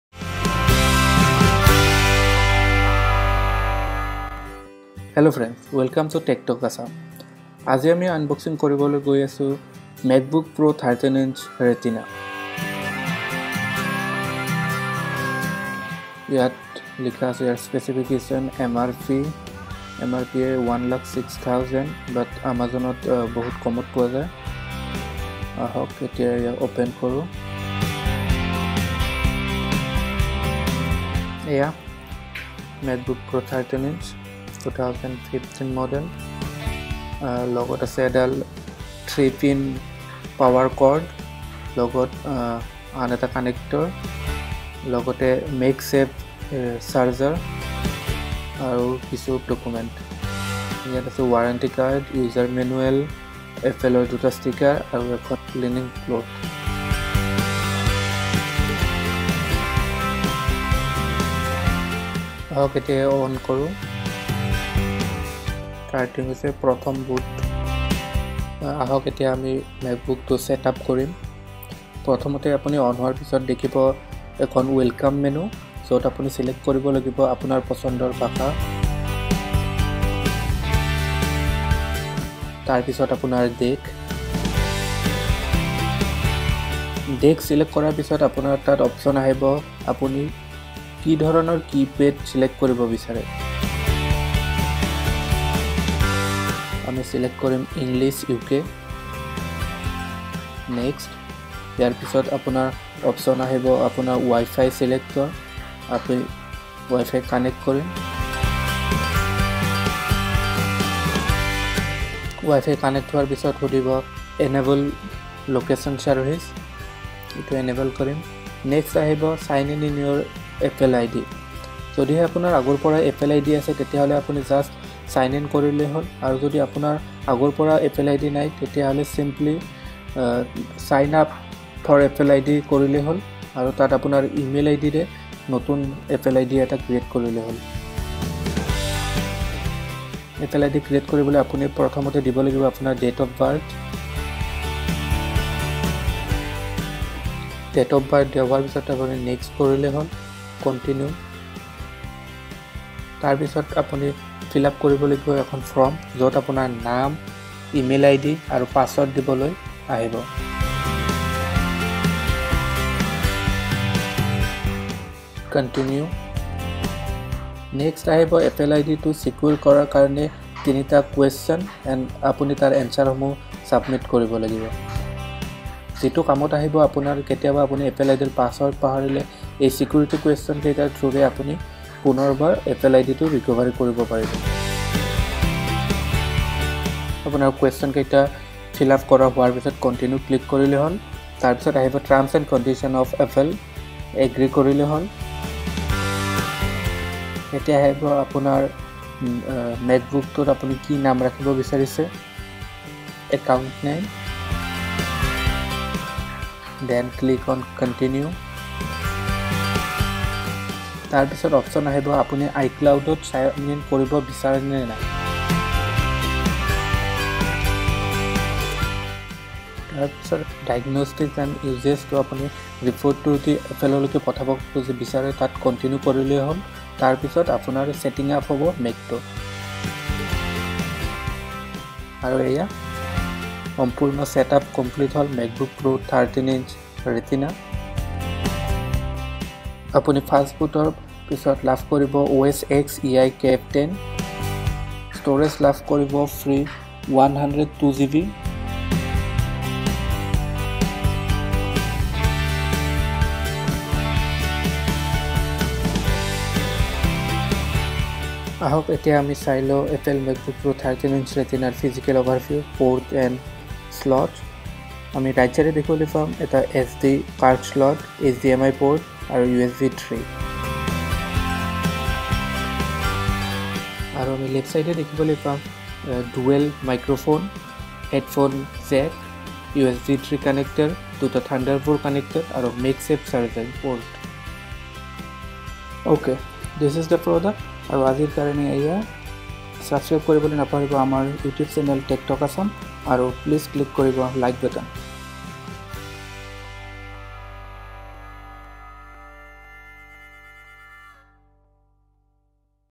Apple Macbook Pro 13 inch Retina |Unboxing & Set up | Assamese
Apple Macbook Pro 13 inch Retina |Unboxing & Set up | Assamese | TechTalk Assam
Follow Us -